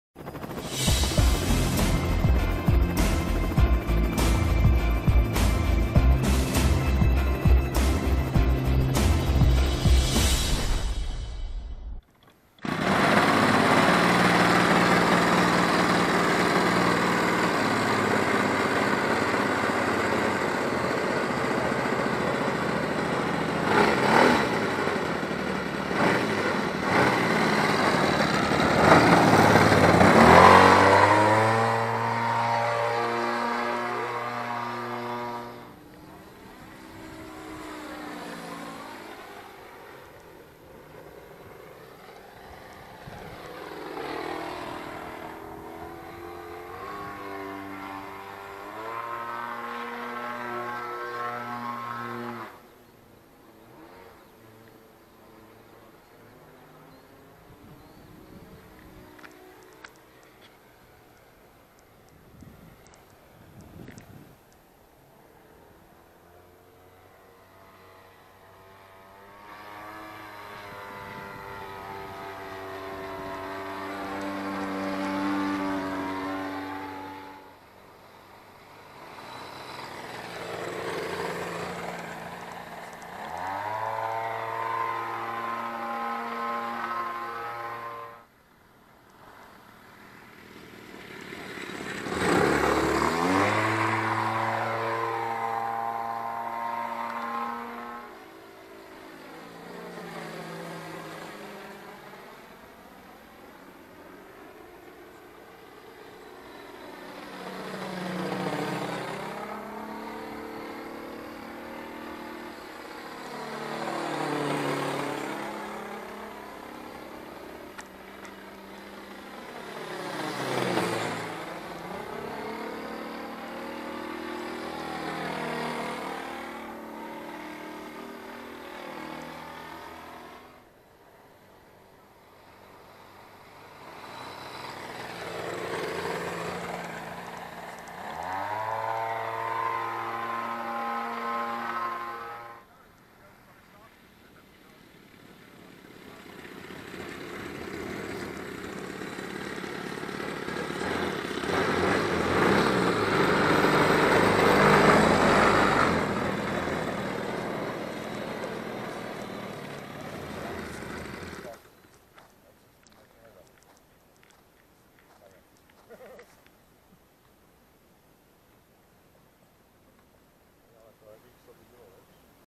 Modeláři letiště Záhoří
RC 3D Akrobat Velox Revolution na letišti Záhoří,pilot Pavel Studeny.